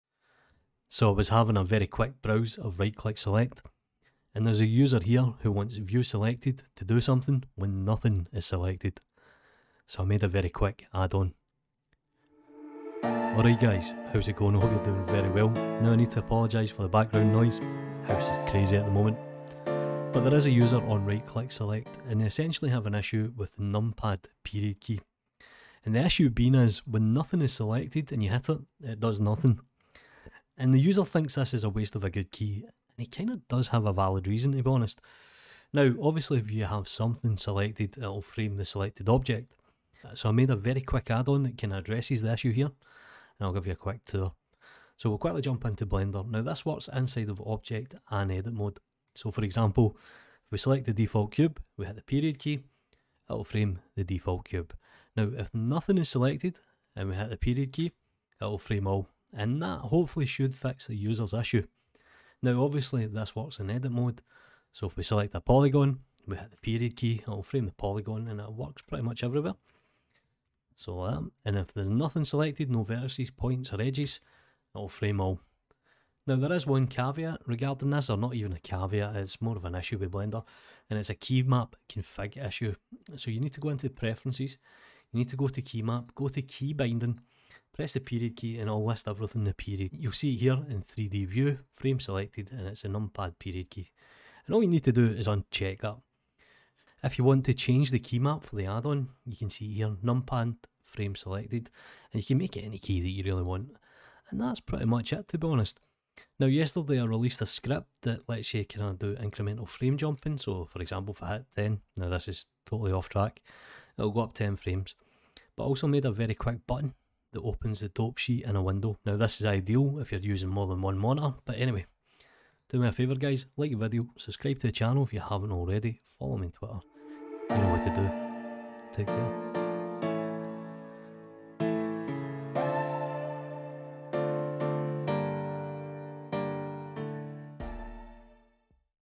Blender add-on: Numpad Frame All / Selected
I was reading a post on RCS so I made a quick addon to address the issue of the numpad period key.